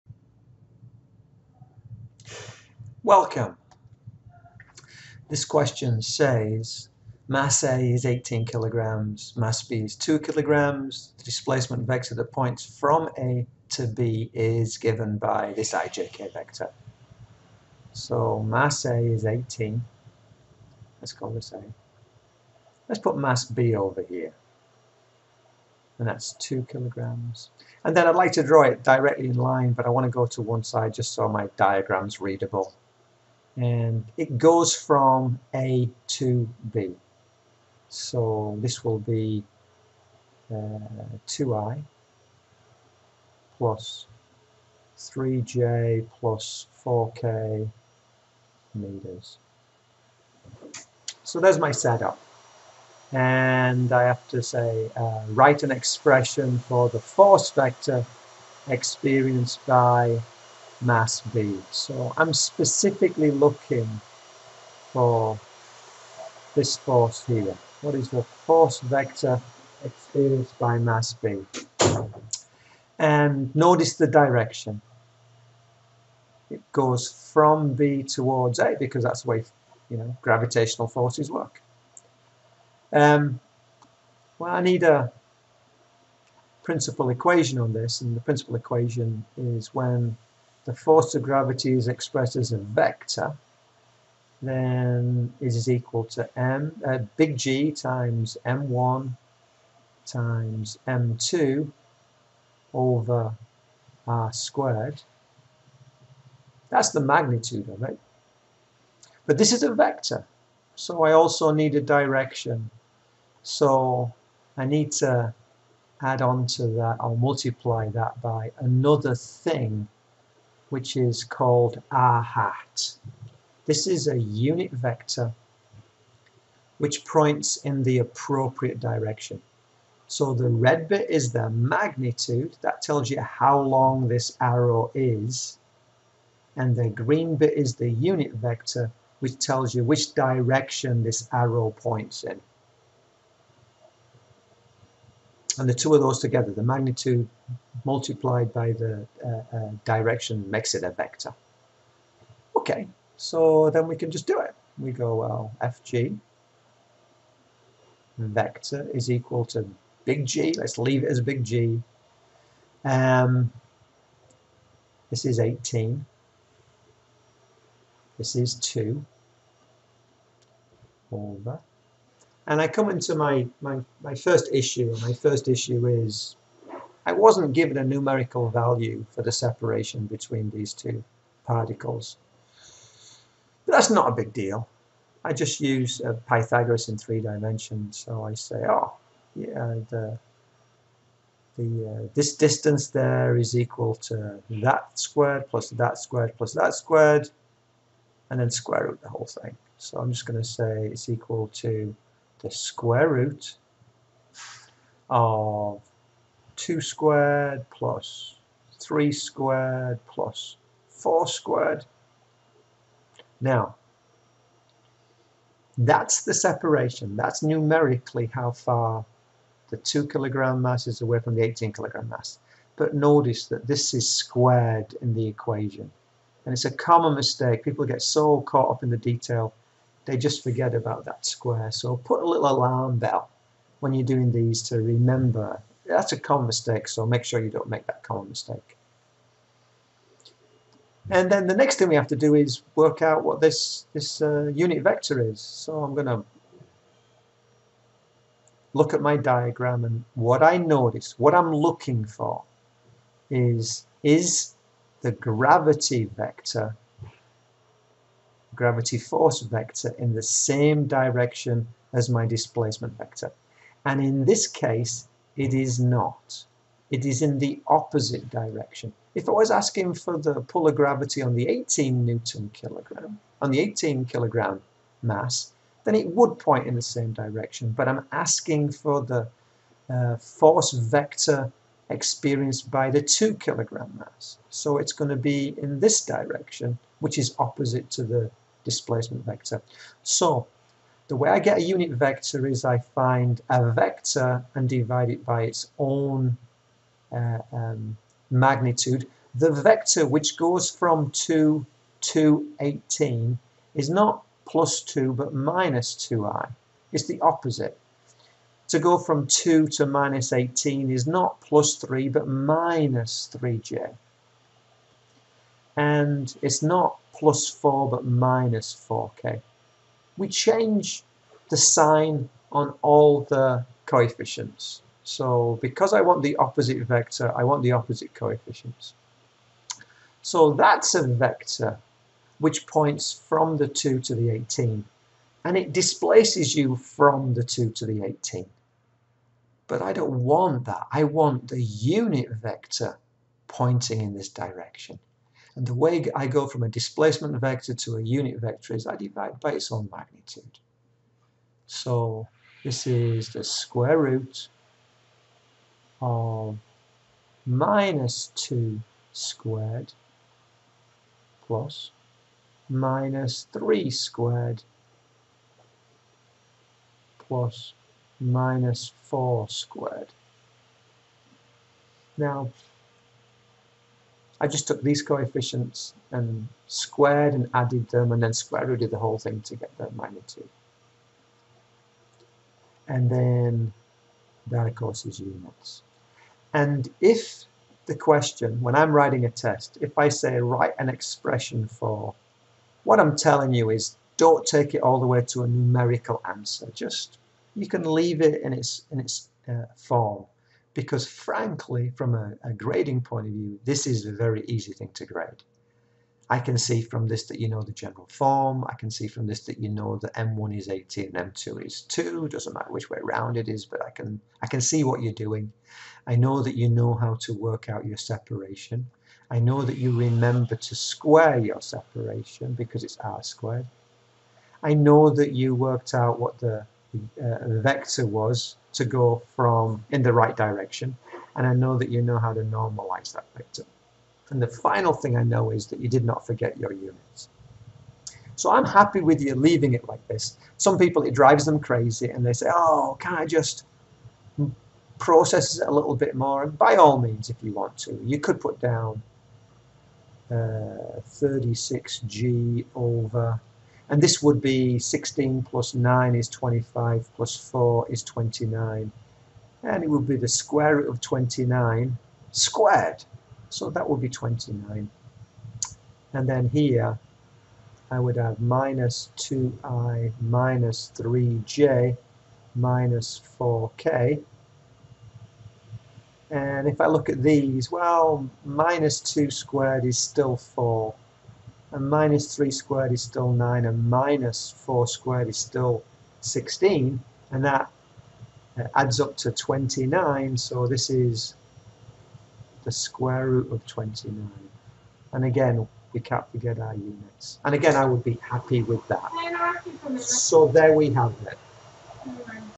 13) Gravitational Force vector 1 - numerical
Find the gravitational force vector given two masses and a displacement vector.